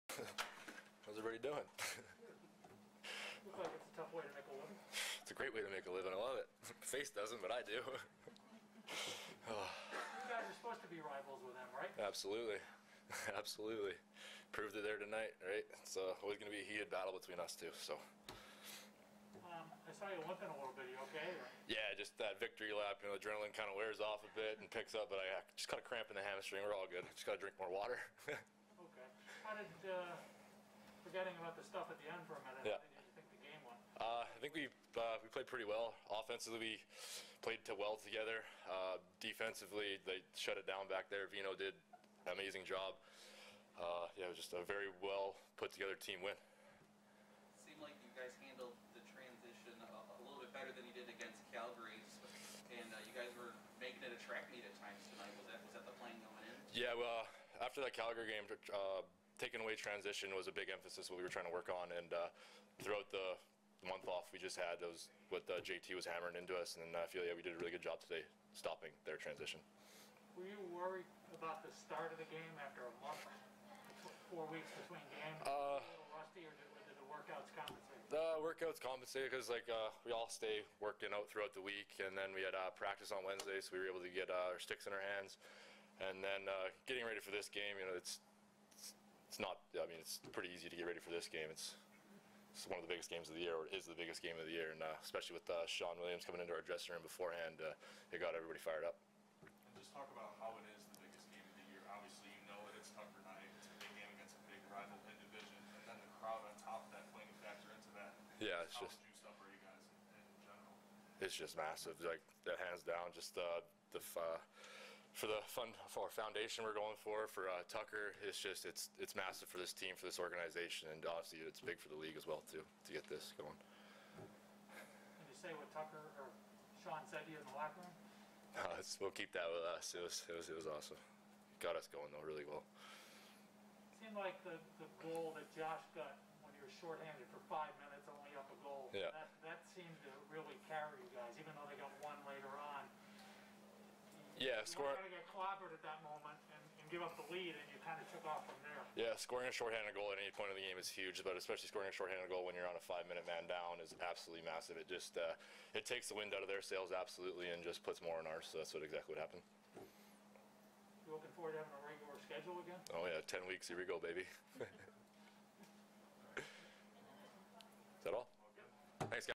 Chase Fraser's Four-Goal Night Against the Toronto Rock | Buffalo Bandits
Buffalo Bandits forward Chase Fraser addressed the media following his four-goal performance in the team's 12-6 win over the Toronto Rock in Buffalo, NY.